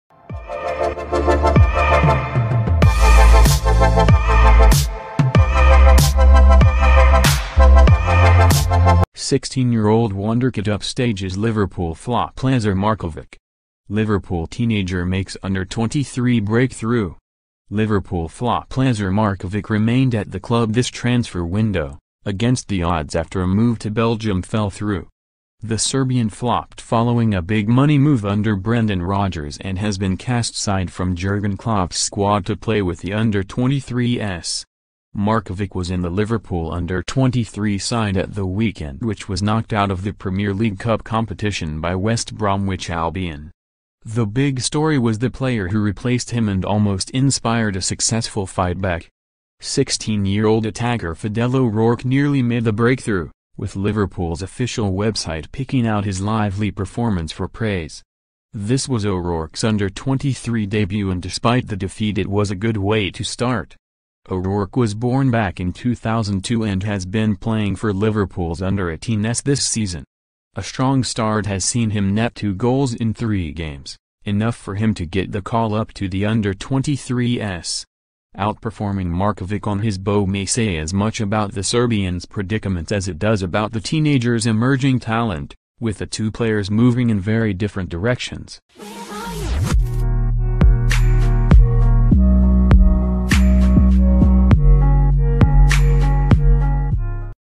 16 year old wonderkid upstages Liverpool flop Lazar Markovic
16-year-old wonderkid upstages Liverpool flop Lazar Markovic. Liverpool teenager makes under-23 break through.

Liverpool flop Lazar Markovic remained at the club this transfer window, against the odds after a move to Belgium fell through.

The Serbian flopped following a big money move under Brendan Rodgers and has been cast side from Jurgen Klopp's squad to play with the under-23s.

Markovic was in the Liverpool under-23 side at the weekend which was knocked out of the Premier League Cup competition by West Bromwich Albion.

The big story was the player who replaced him and almost inspired a successful fightback.

16-year-old attacker Fidel O'Rourke nearly made the breakthrough, with Liverpool's official website picking out his 'lively' performance for praise.

This was O'Rourke's under-23 debut and despite the defeat it was a good way to start.

O'Rourke was born back in 2002 and has been playing for Liverpool's under-18s this season.

A strong start has seen him net two goals in three games, enough for him to get the call up to the under-23s.

Outperforming Markovic on his bow may say as much about the Serbian's predicament as it does about the teenager's emerging talent, with the two players moving in very different directions.